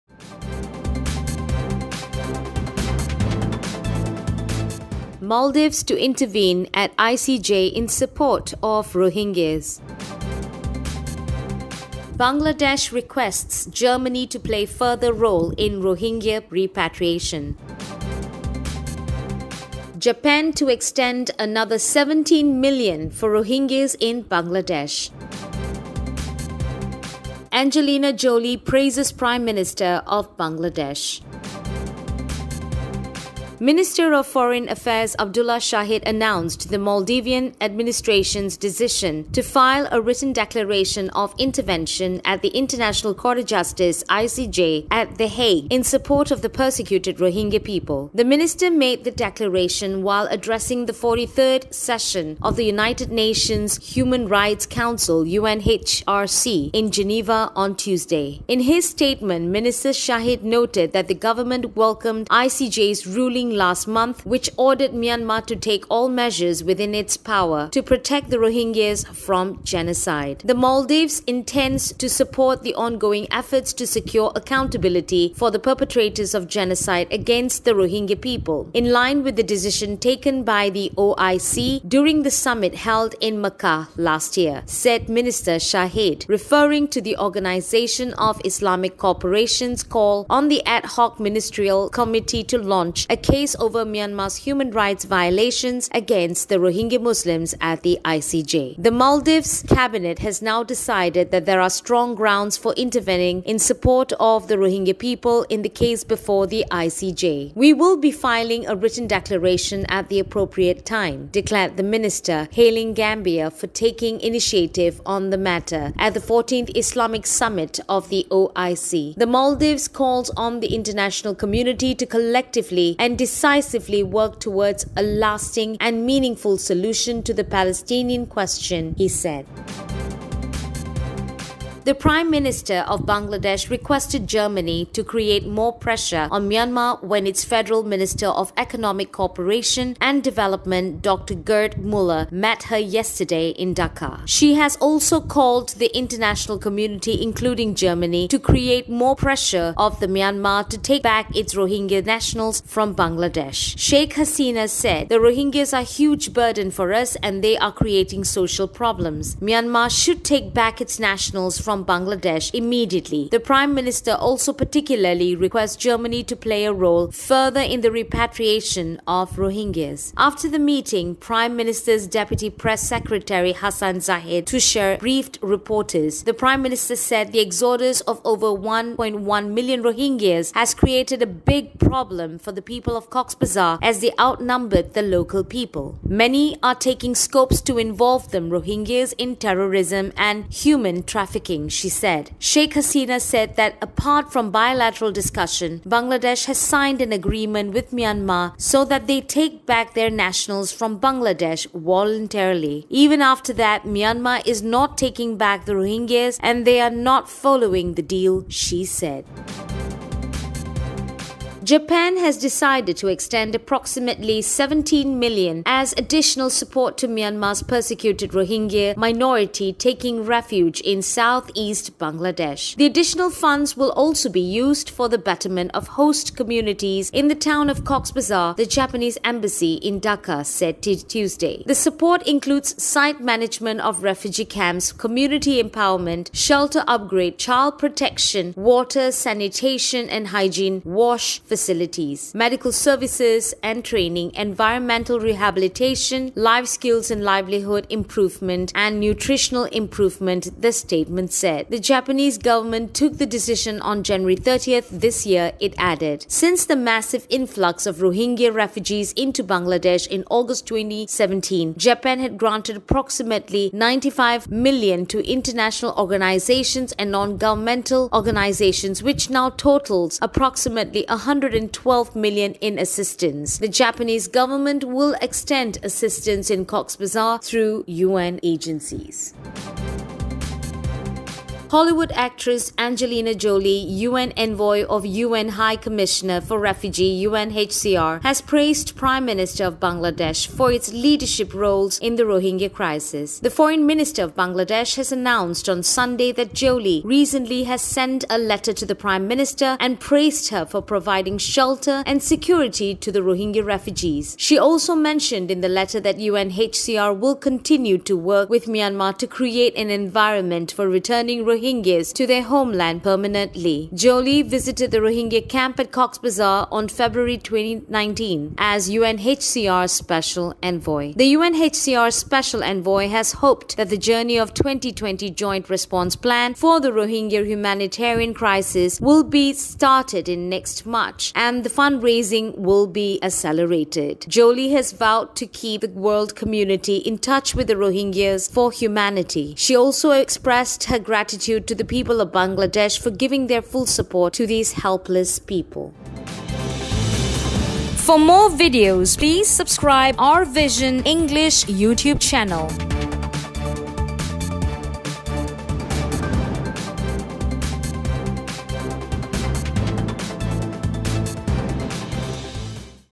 Bangladesh Requests Germany to Play Further Role in Rohingya Repatriation
Maldives to Intervene at ICJ in Support of Rohingya
Japan to extend another $17M for Rohingya in Bangladesh
Angelina Jolie Praises Prime Minister of Bangladesh